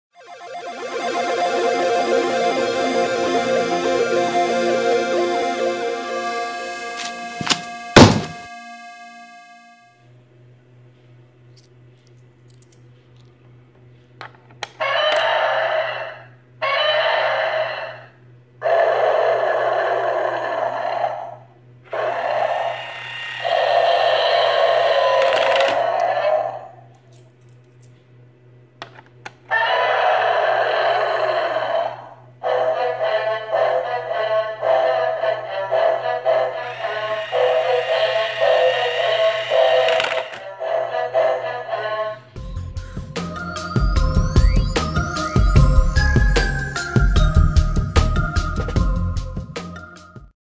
Godzilla Coin Stealing Money Bank
No one is safe from Godzilla!

This mechanical money bank makes saving fun for everyone!

Place your coin on the blue plate behind the boat, and listen as Godzilla roars. Suspense-filled music will fill the air!